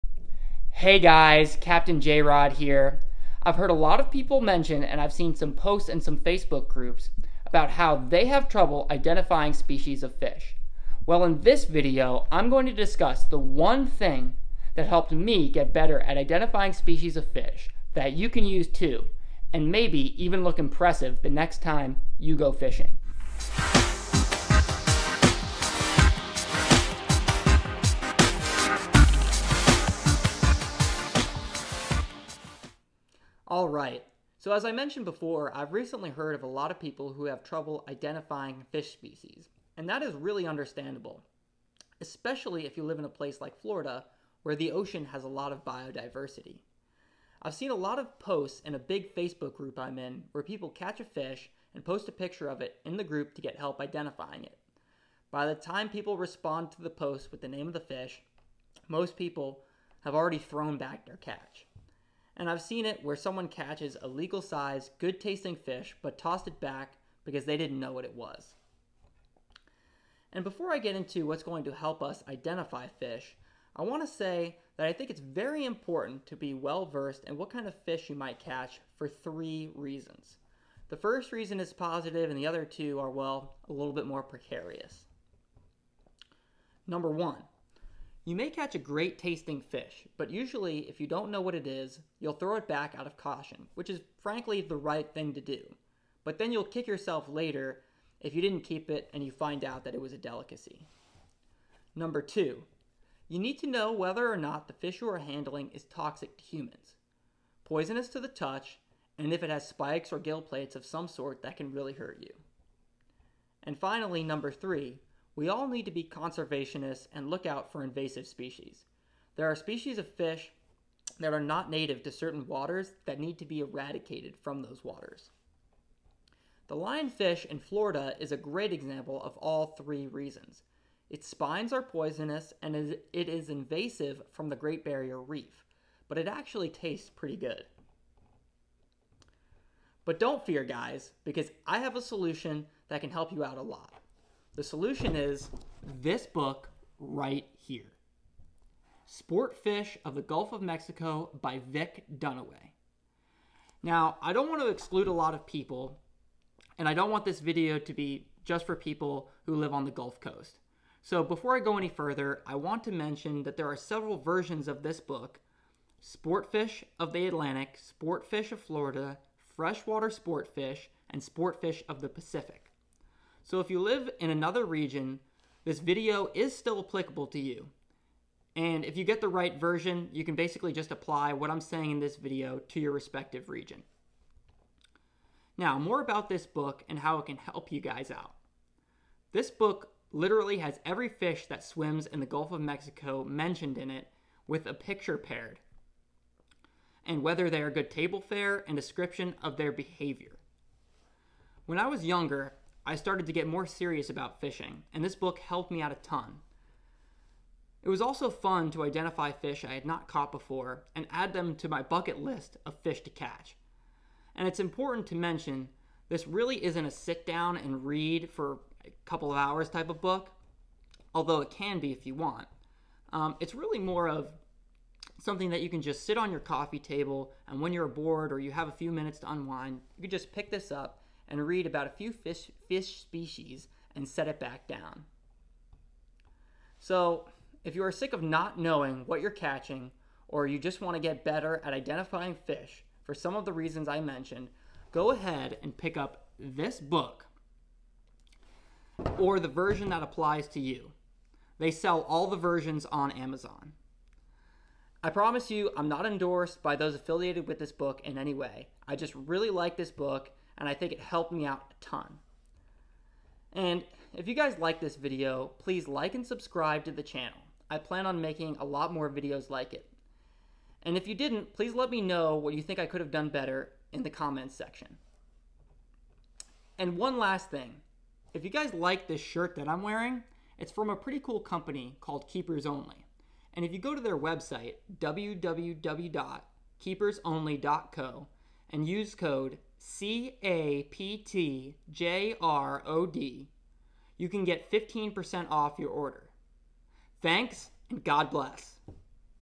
How to IDENTIFY FISH
In this video, I discuss the one thing that helped me identify different species of fish better.

Link to book mentioned in video

*Opinions on this channel are my own.*